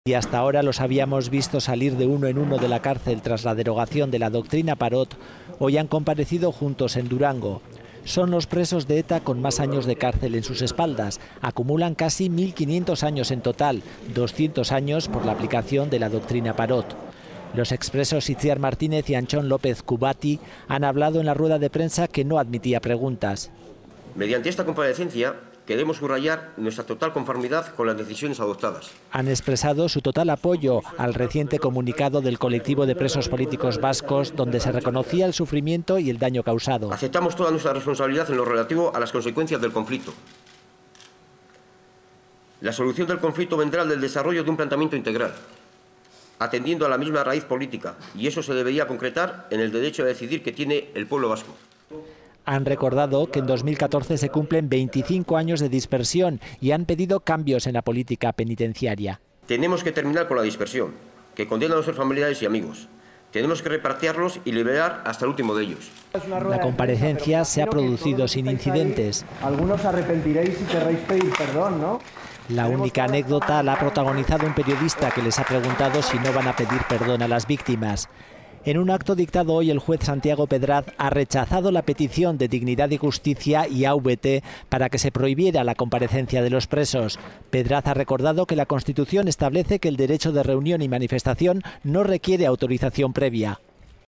Los expresos de ETA reconocen su responsabilidad en el conflicto
En el acto celebrado en el Kafe Antzokia, los expresos participantes han leído dos comunicados y se han adherido al comunicado presentado por el EPPK el pasado 28 de diciembre.